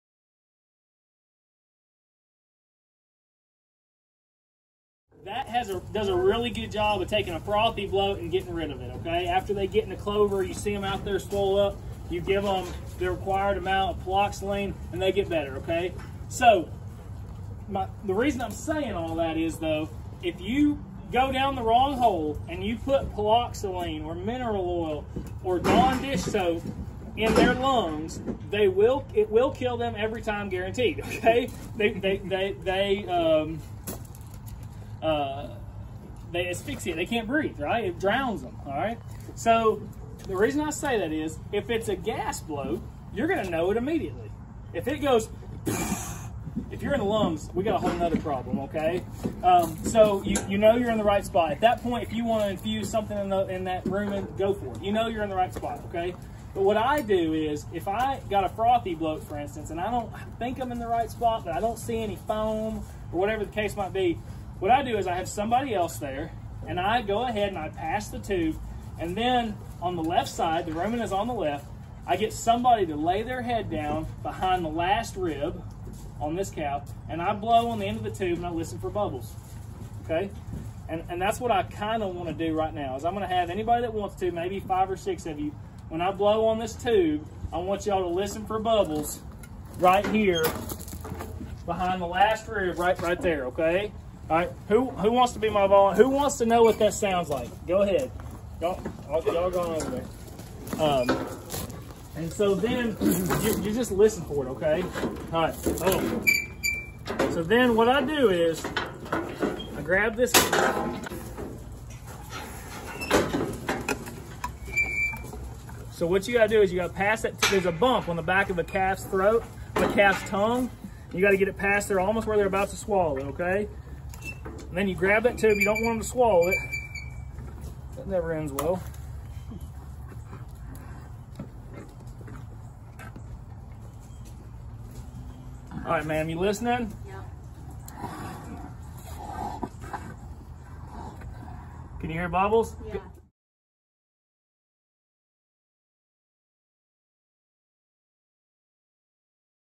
How to Pass a Tube into the Rumen
UT Williamson County Extension’s Master Beef Class had Dr. Martin Sander come out and discuss common vet emergencies including calving dystocia and bloating.